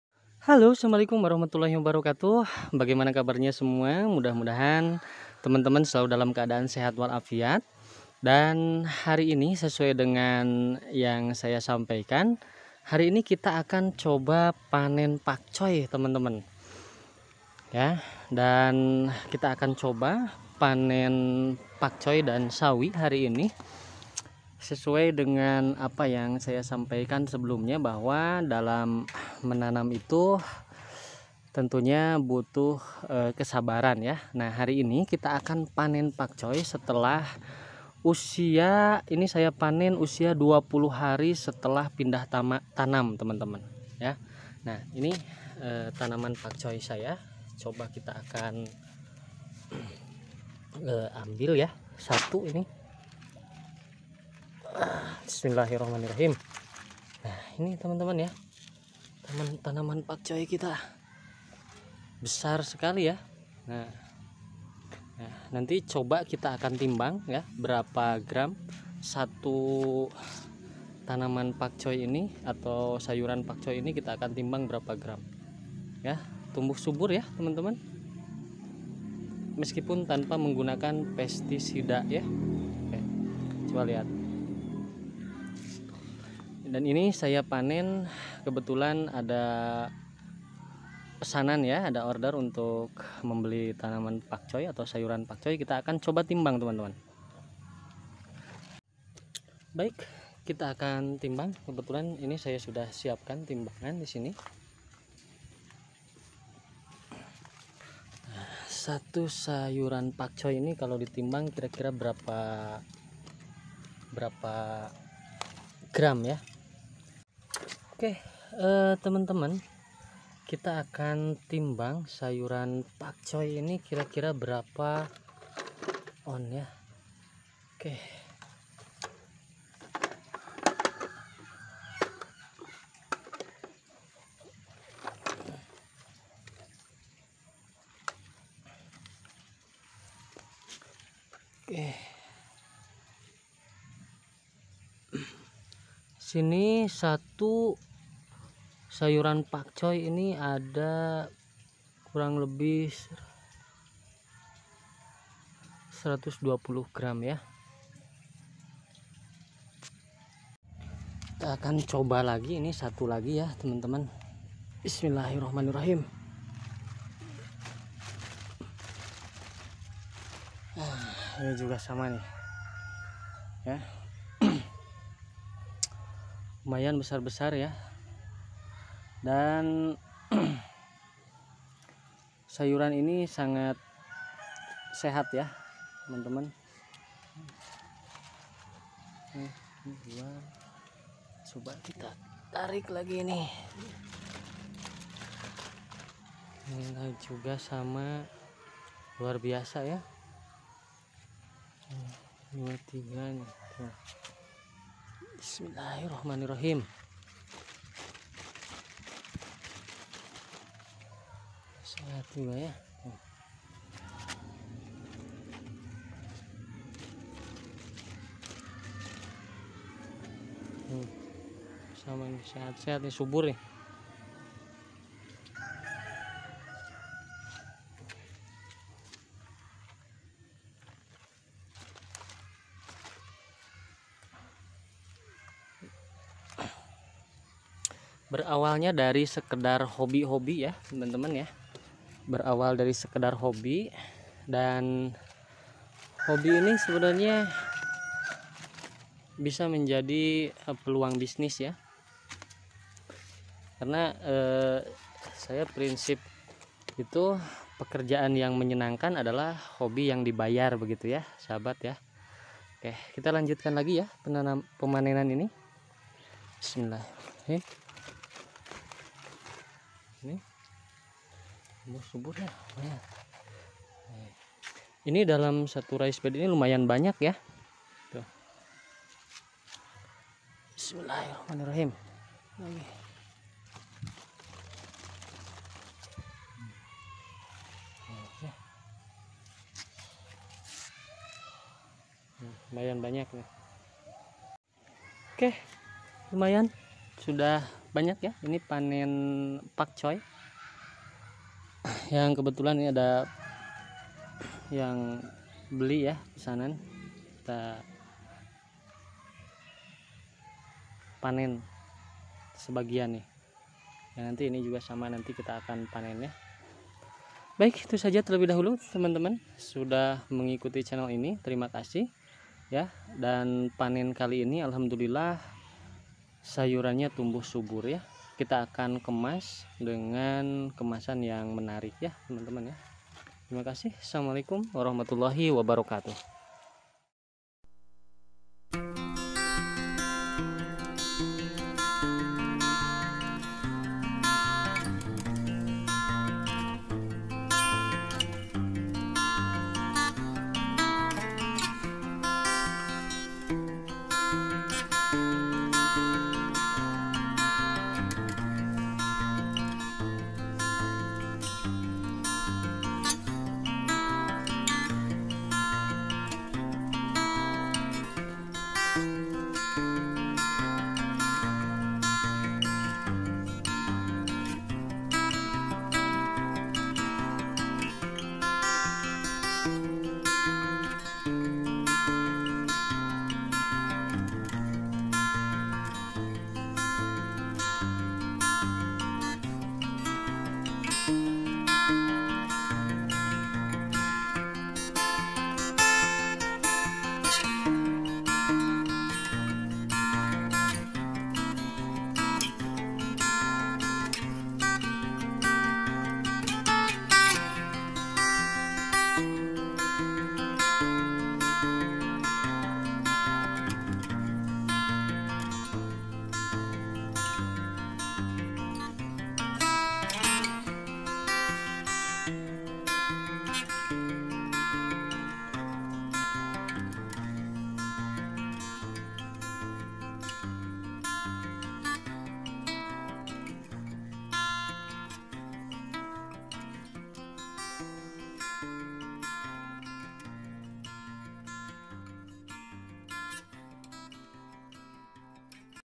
BERKEBUN DI PEKARANGAN RUMAH || PANEN SAYURAN SEGAR
assalamualaikum..di video kali ini saya berbagi pengalaman panen sayuran di kebun saya di usia 20 HST tanpa rekayasa